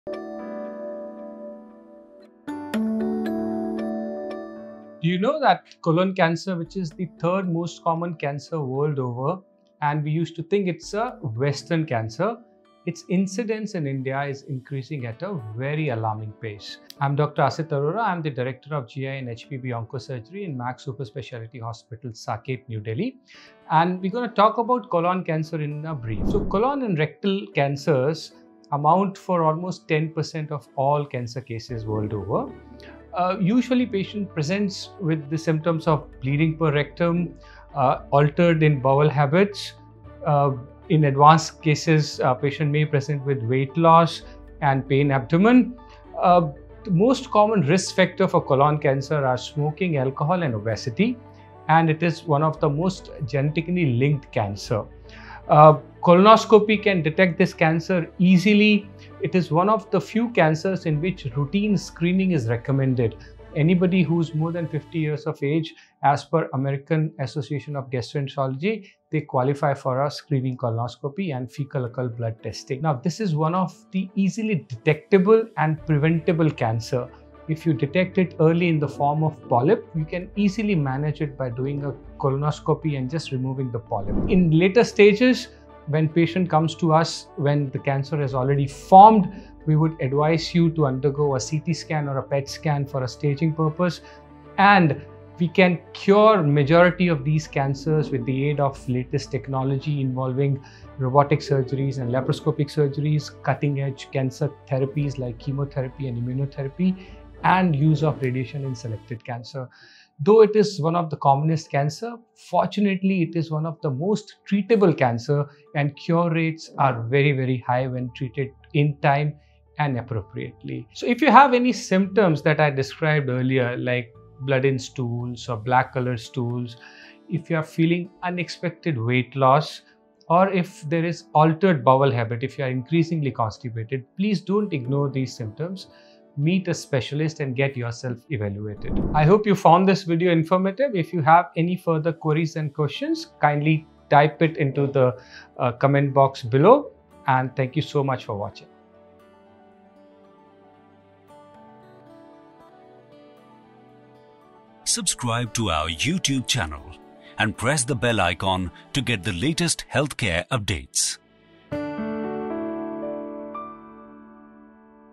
Colorectal Cancer: Incidence and Screening │ Dr. Asit Arora │ Max Hospital, Saket
The typical symptoms of Colorectal Cancer are rectal bleeding, abdominal pain, and altered bowel habits. A colonoscopy is an effective screening procedure which detects and facilitates the timely treatment of cancer. Dr. Asit Arora, Director, Surgical Oncology, Gastrointestinal & Hepatobiliary Oncology, Max Hospital, Saket, Delhi, India, explains the screening and treatment protocols for a successful cure.